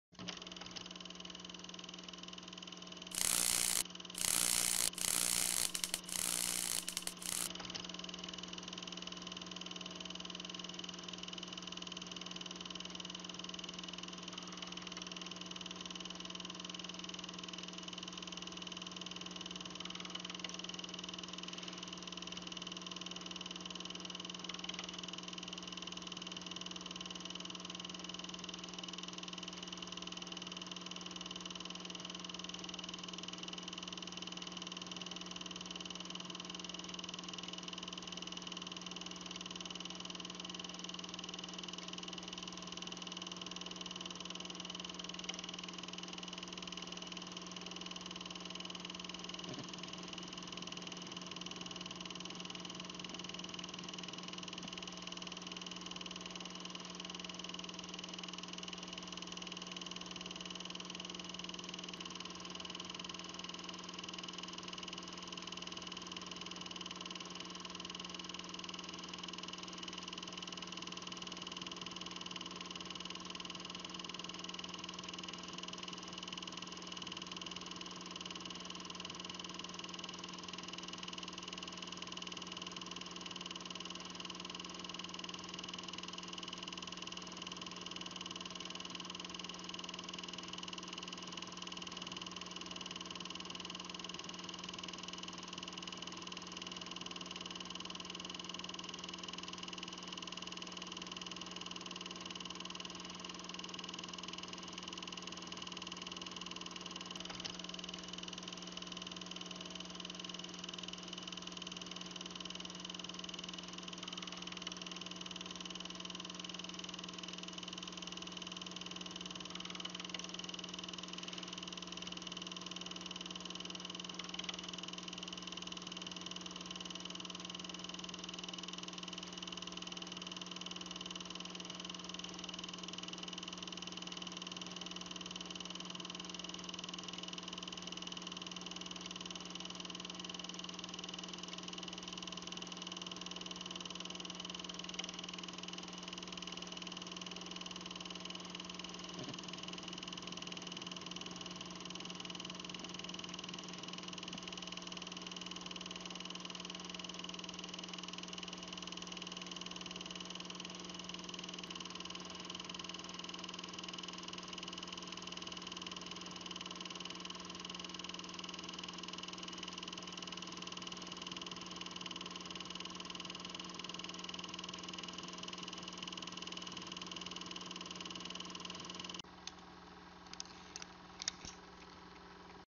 8mm Home Movie Kids Running Around and People Swimming
This 8mm home movie reel has some kids running around in the yard. They seem to be having fun while the adults tend to other stuff. The family then enjoys a swim in the above ground pool.